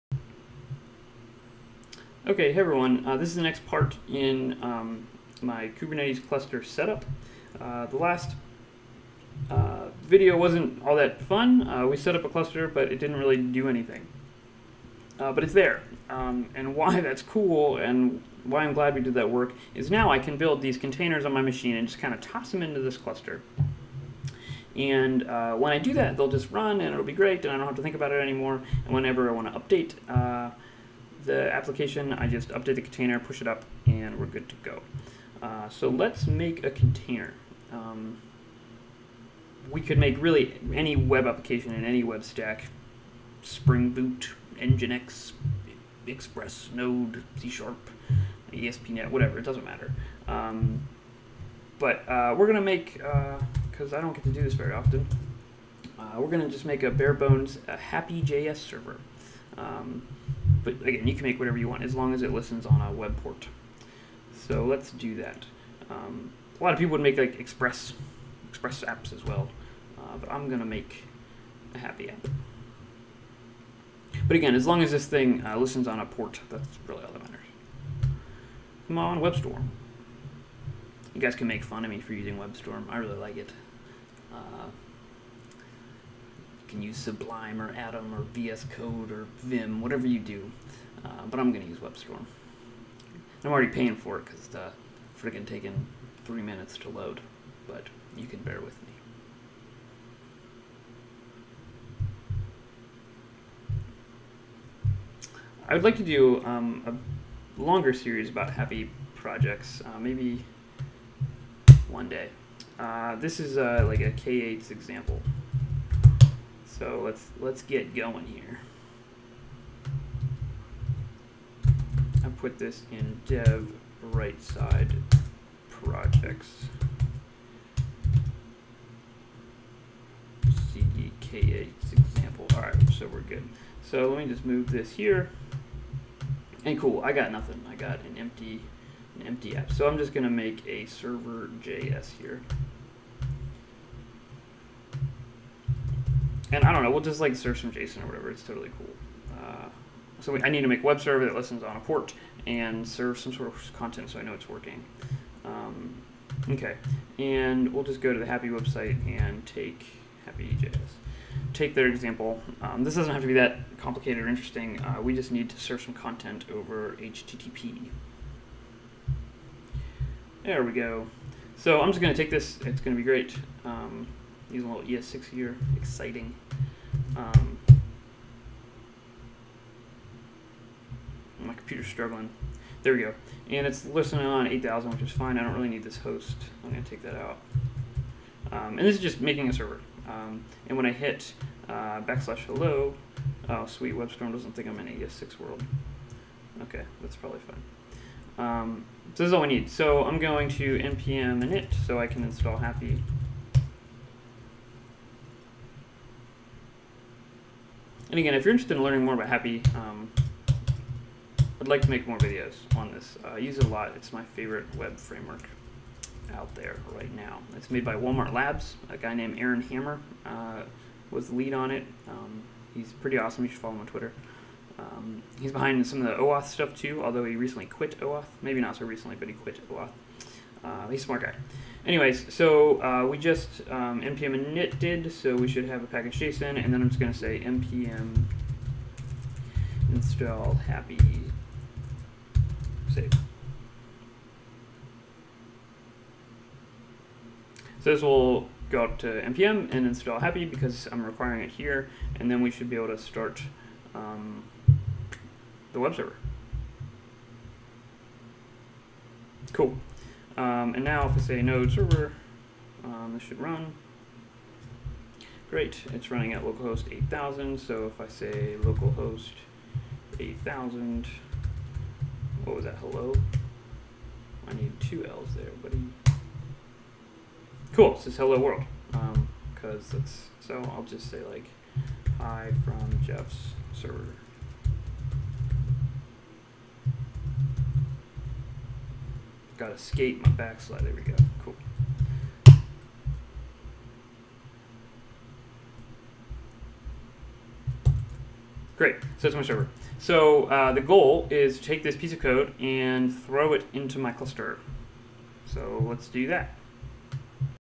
(Part 4) Getting Started with Kubernetes on AWS - Hapi.js webserver
In this video, I make a simple Hapi.js server. This is the server I plan on placing into a container and deploying into Kubernetes.